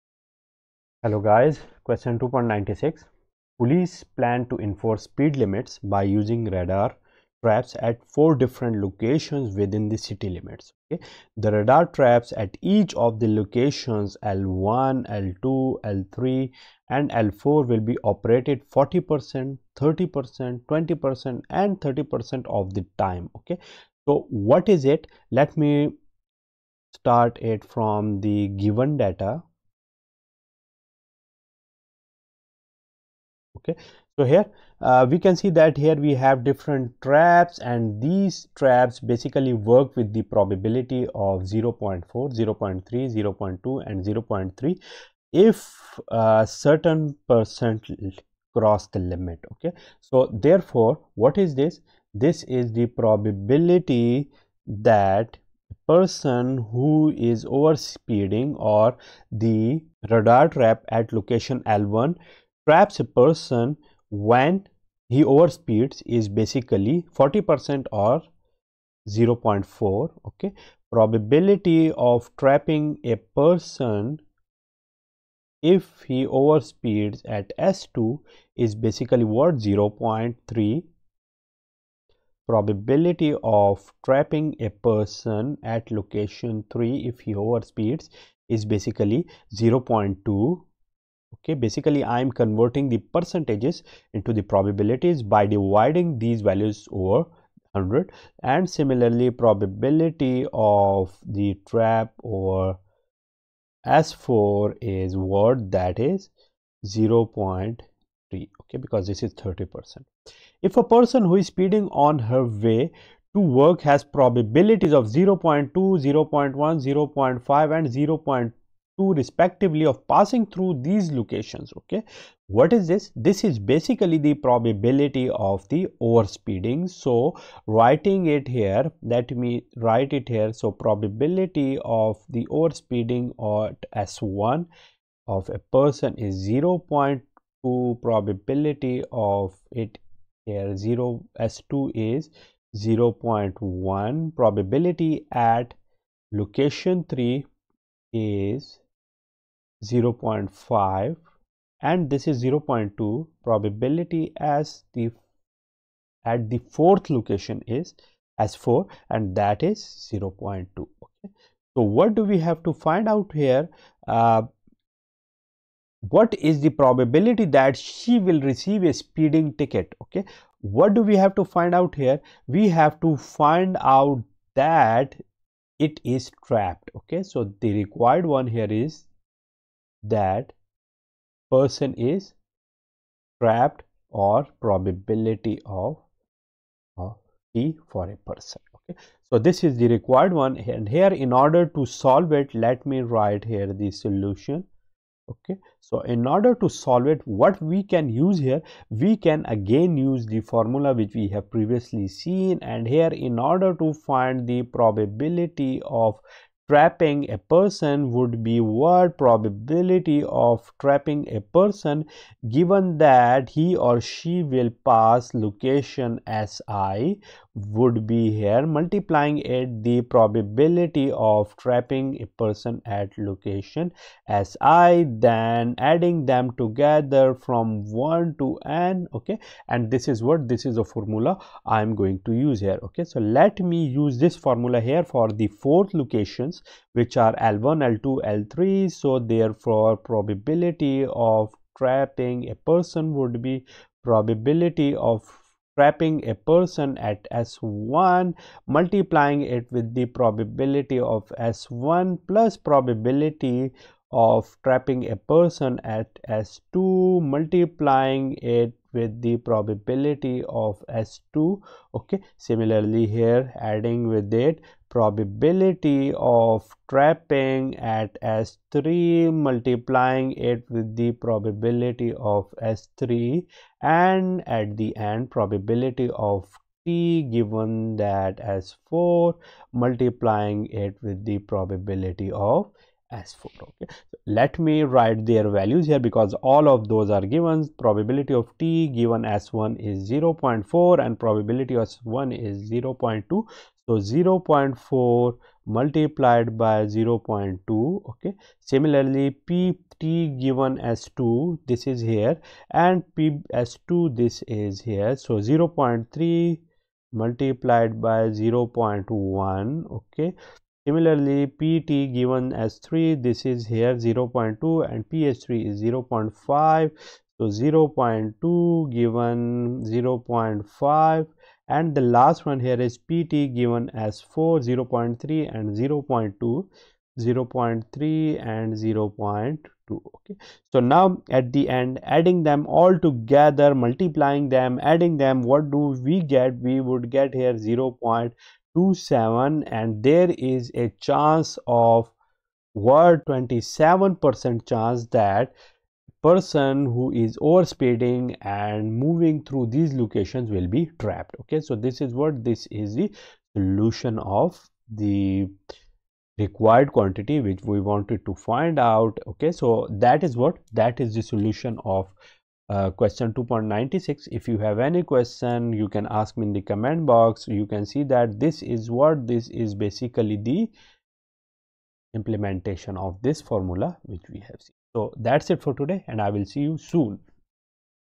Probability & Statistics for Engineers & Scientists by Walpole | Solution Chap 2
This problem is related to conditional probability, which targets the solution of a problem involving two or more events interrelated with each other

2.96 Police plan to enforce speed limits by using radar traps at four different locations within the city limits. The radar traps at each of the locations L1, L2, L3, and L4 will be operated 40%, 30%, 20%, and 30% of the time. If a person who is speeding on her way to work has probabilities of 0.2, 0.1, 0.5, and 0.2, respectively, of passing through these locations, what is the probability that she will receive a speeding ticket?

Happy Learning :-)